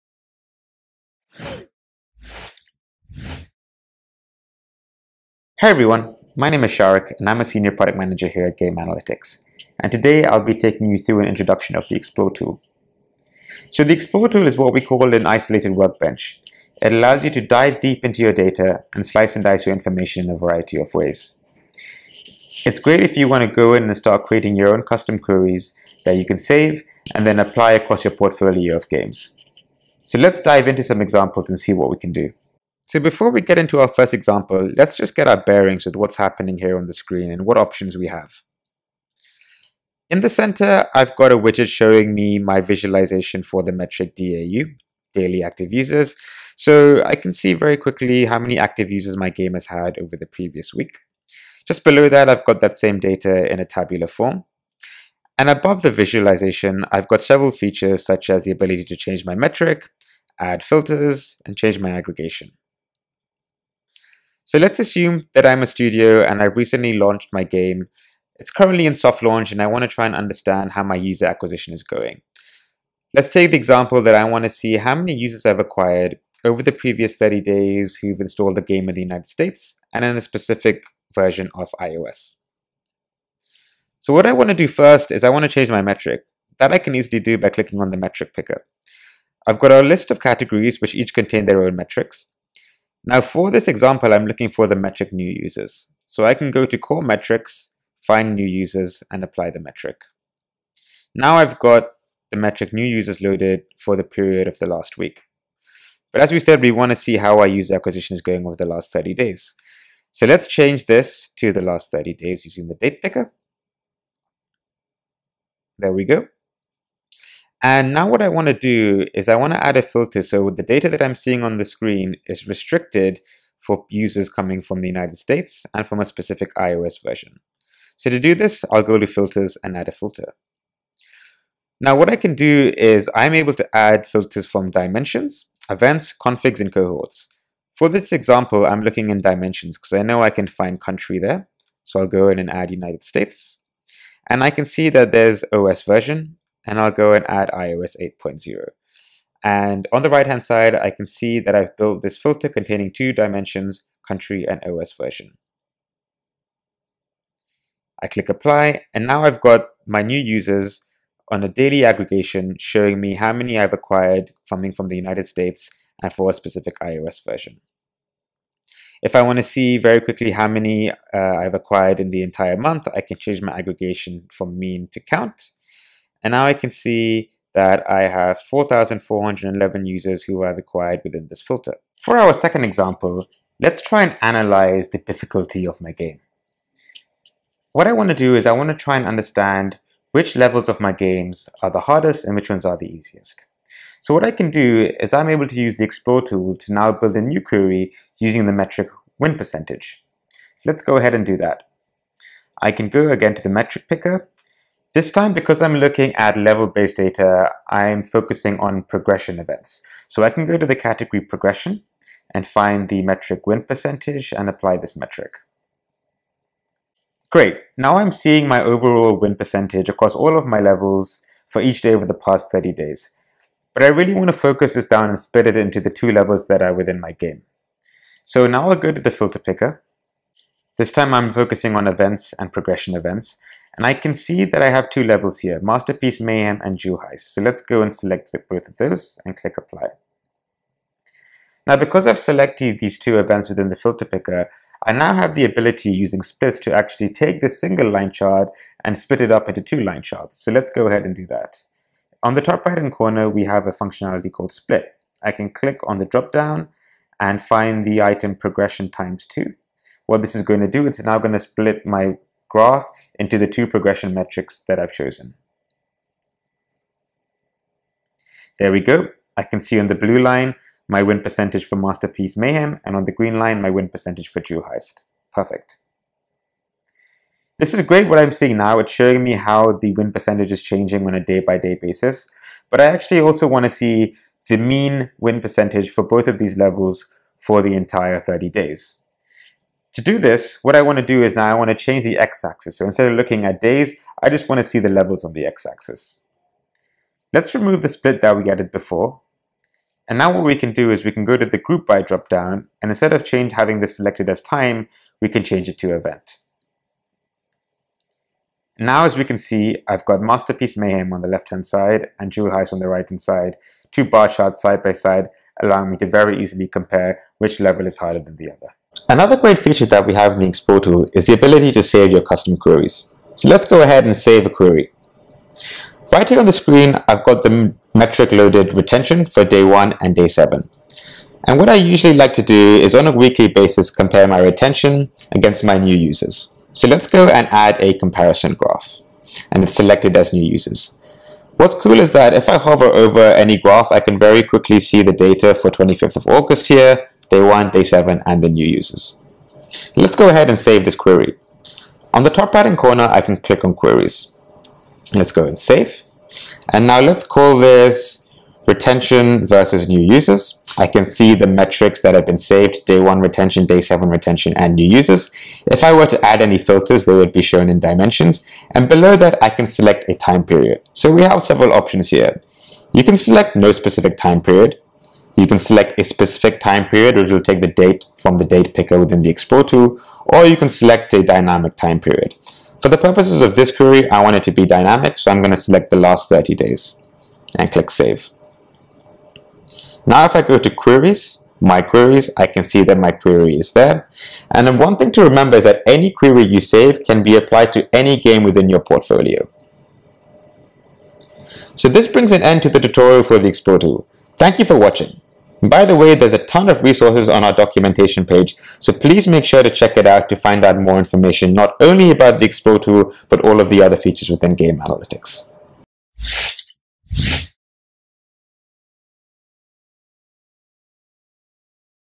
GameAnalytics - Explore Tool Overview
Here's a quick walkthrough of the Explore tool in the GameAnalytics platform.

For more information on all of our other features, please visit our documentation